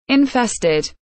How to pronounce Infested / Infested pronunciation
Raccoon teaches you how to pronounce Infested . Raccoon vous apprends a prononcer Infested . Como pronunciar Infested